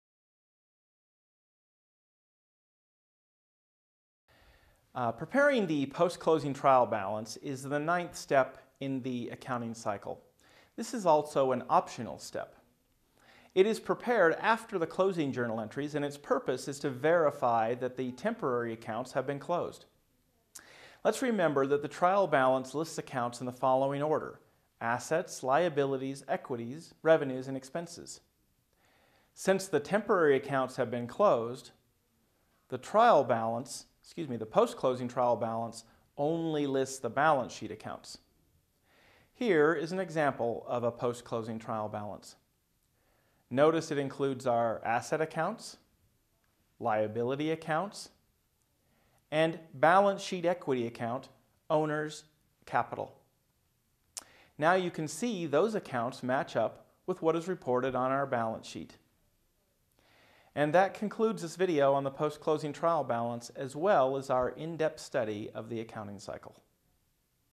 Accounting Cycle Step 9: Post-Closing Trial Balance for Sole Proprietorships
The final step in the accounting cycle is this option step: prepare a post-closing trial balance.  Since the income statement and statement of owner's equity accounts have been closed, the post-closing trial balance should only include balance sheet accounts.